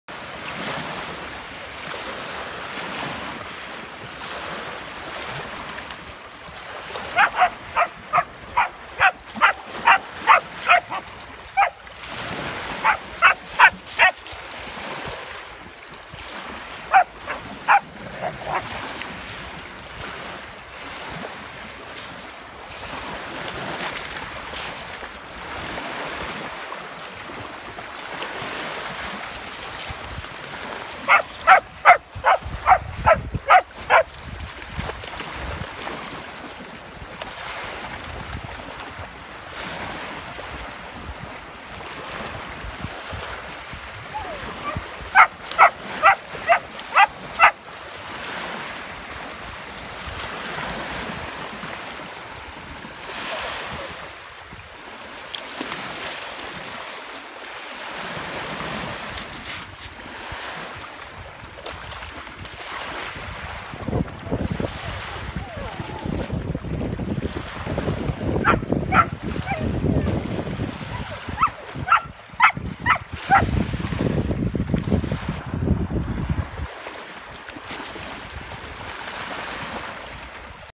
Swimming dogs in Lake Geneva
Harold the yellow labrador; Mia the Portuguese Water Dog; and Knight the Samoyed paddling in Lake Geneva, Switzerland